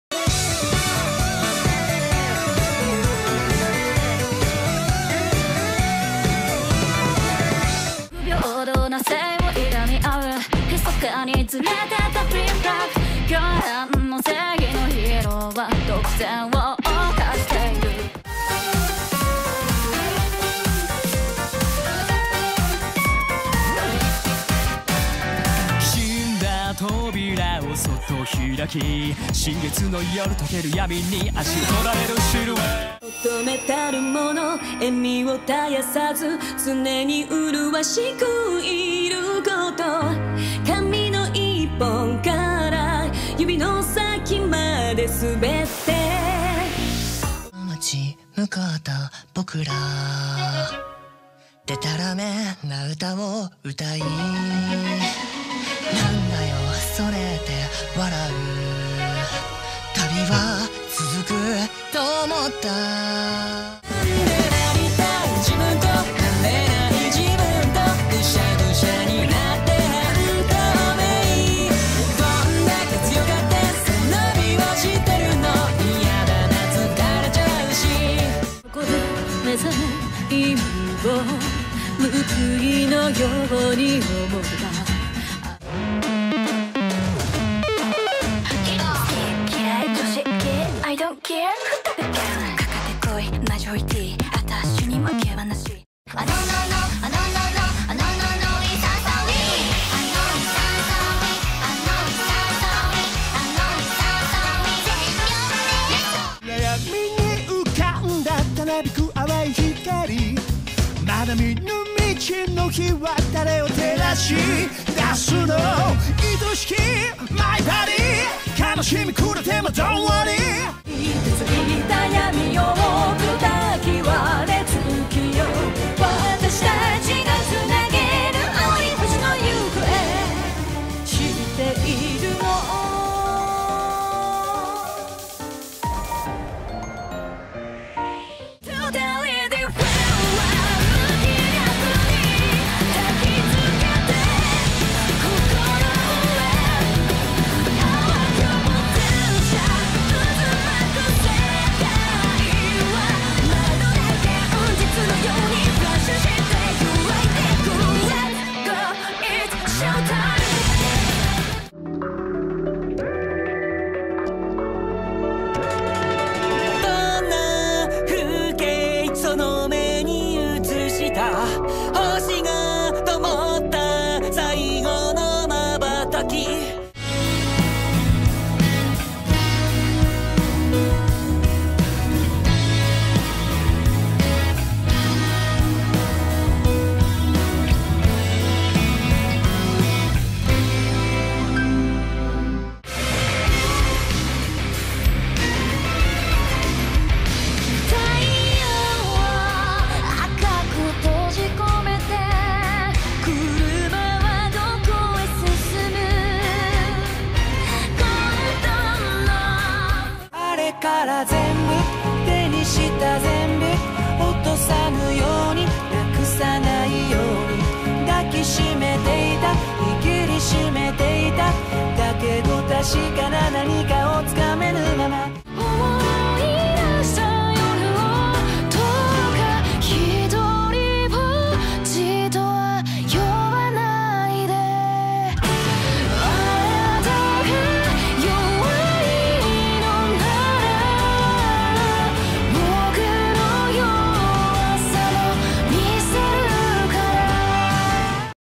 Top anime fall 2021
If your favorite anime aren't in the top, it means that I haven't watched them, and I don't know whether they are good or bad.